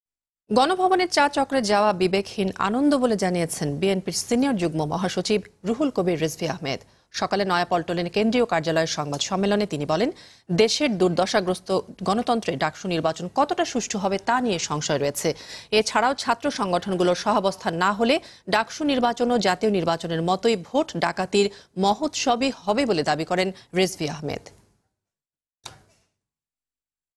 গণভবনের চা চক্রে যাওয়া বিবেকহীন আনন্দ-রুহুল কবির রিজভী
News : গণভবনের চা চক্রে যাওয়া বিবেকহীন আনন্দ-রুহুল কবির রিজভী

- Team RTV